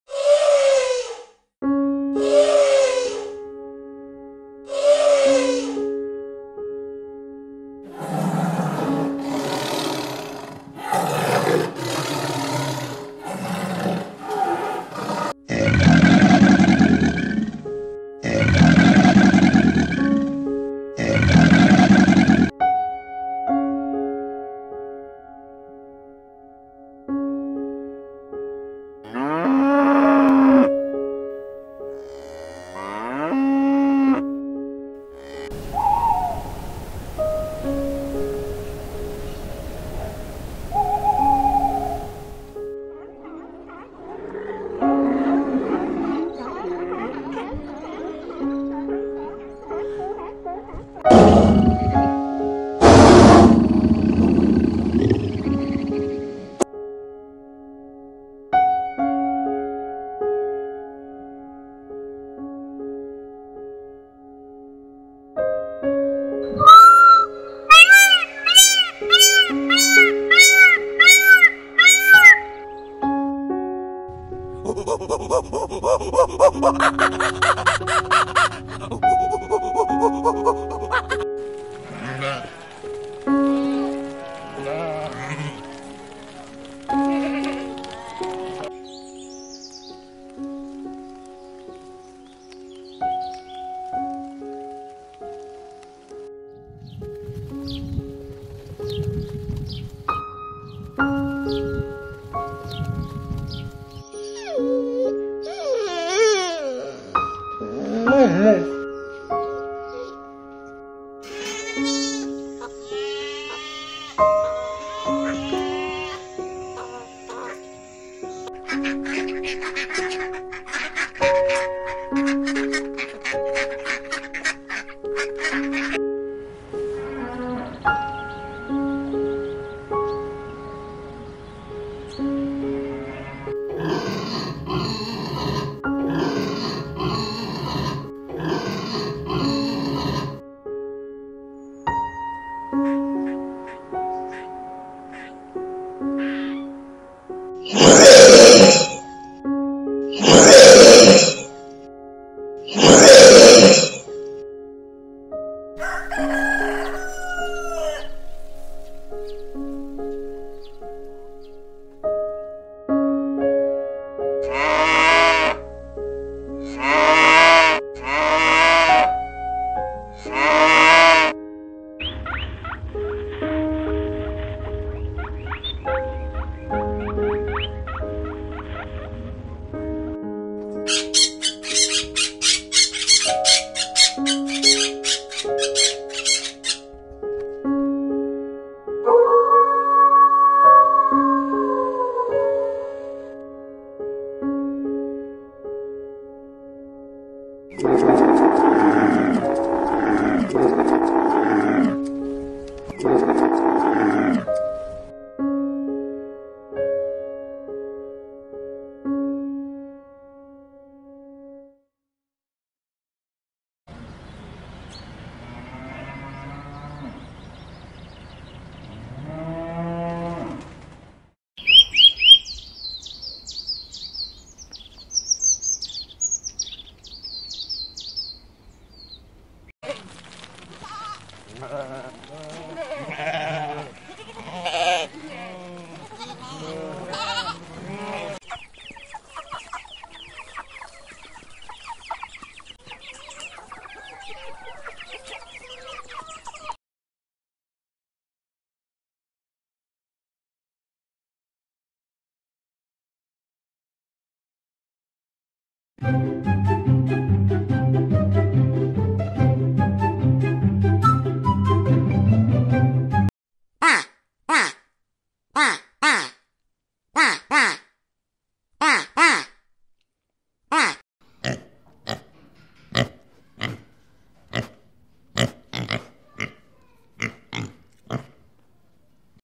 Bustling animal world sounds around us - Duck, Chicken, Goat, Pig, Bear, Camel, Moose, Cat, Dog
In this video, we will be taking a look and listening some of amazing animal sounds and cute voices in the Wild, from horses neighing, sheeps bleating, cows mooing, pigs snorting, donkeys braying, roosters crowing, cats meowing, dog barking, chickens clucking, and more incredible animal sounds and voices in the Wild.

Watch and learn about the different sounds these animals make and enjoy some of the funny animal moments you will ever see.